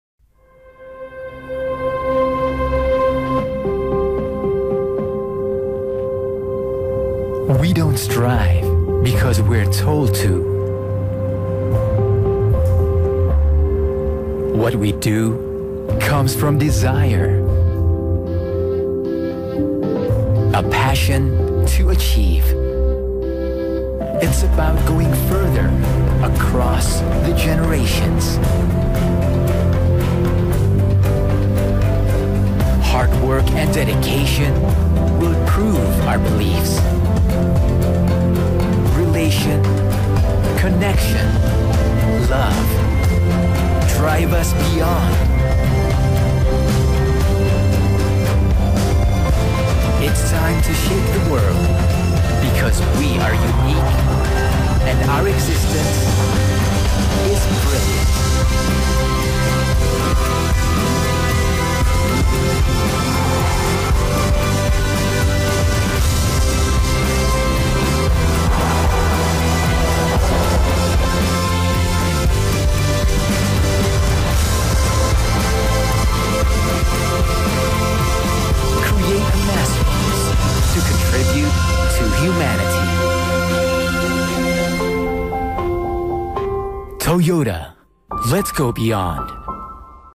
Iklan Toyota - Let's Go Beyond Profile 90sec (2015)
TVC ini ditampilkan dengan tujuan mengganti slogan "Looking Forward" dengan slogan baru "Let's Go Beyond".